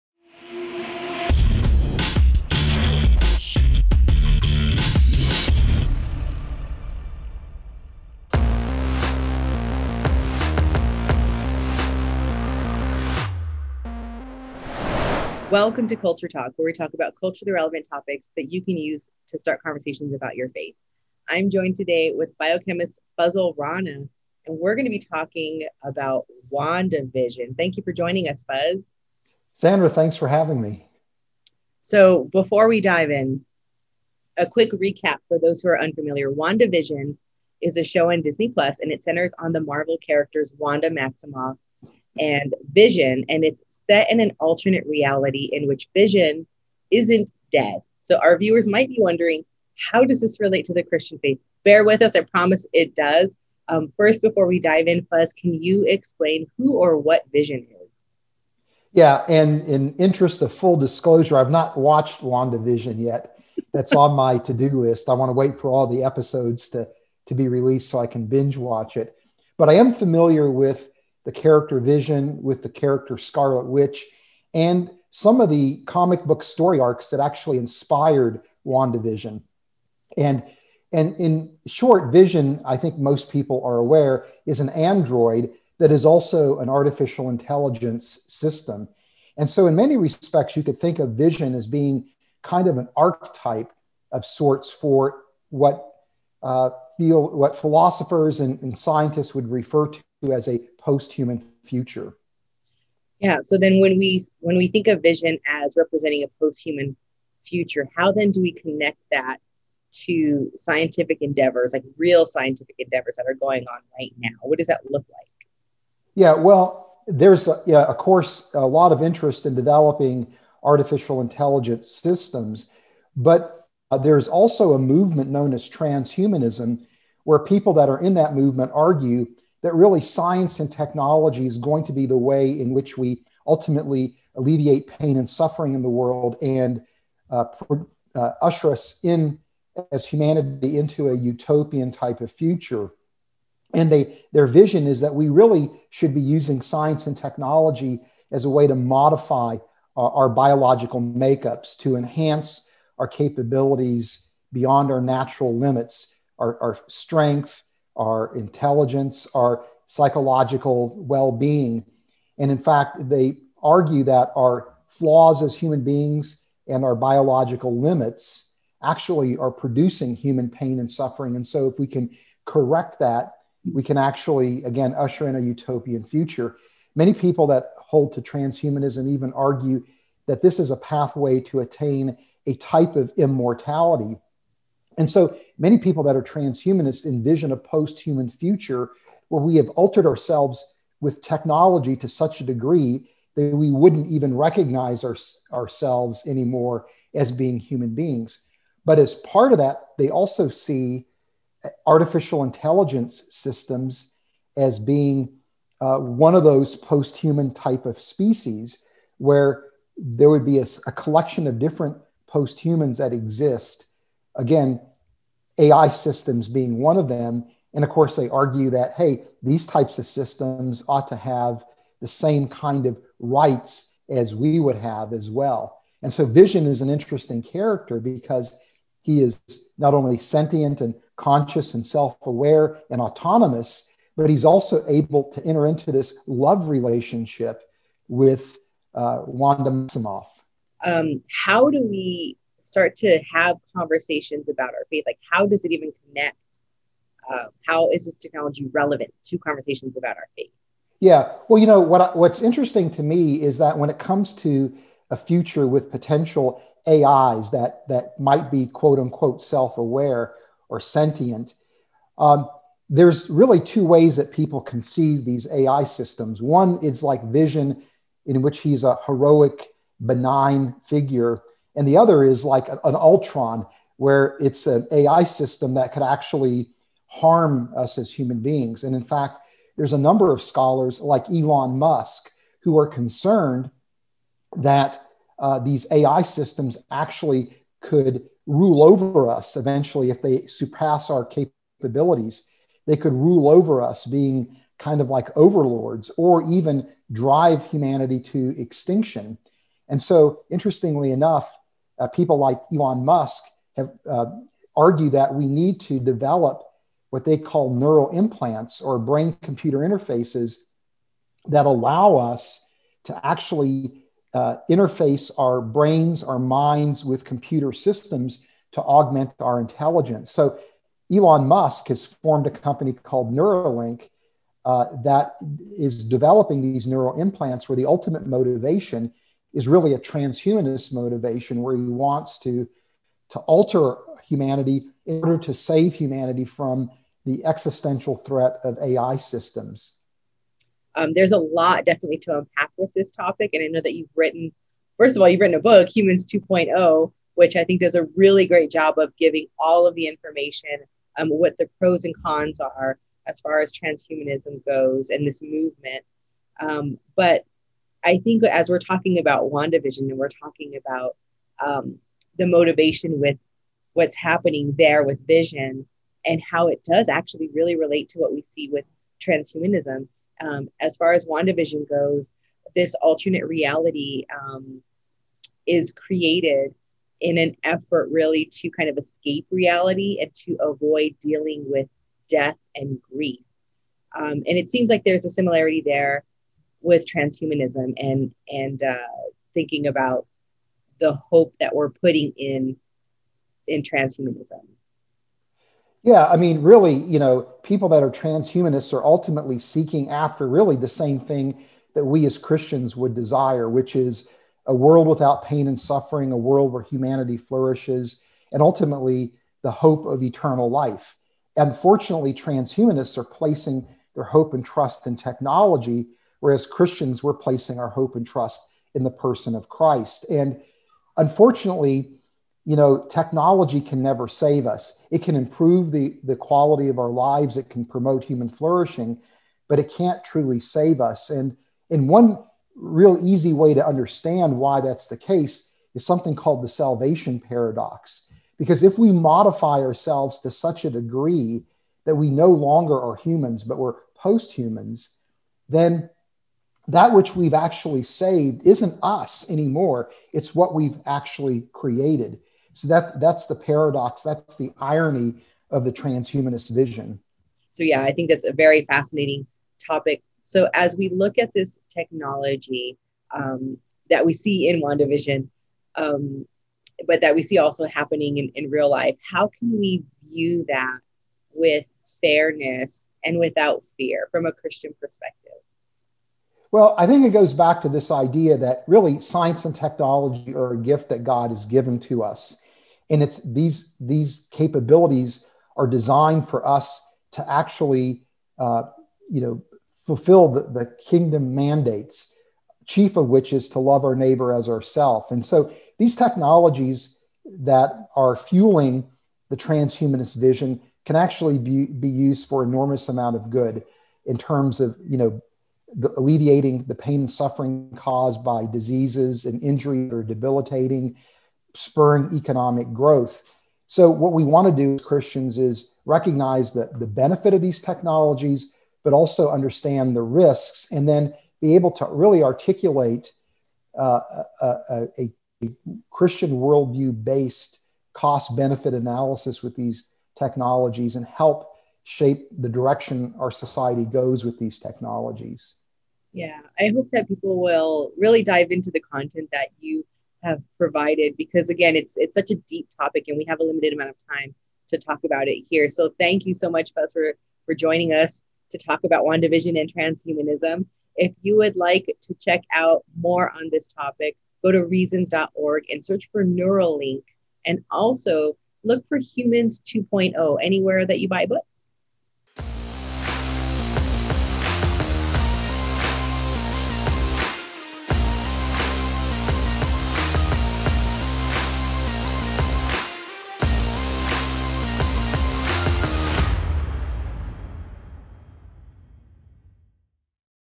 Culture Talk: WandaVision and the Transhumanist Movement
With biochemist Fazala Rana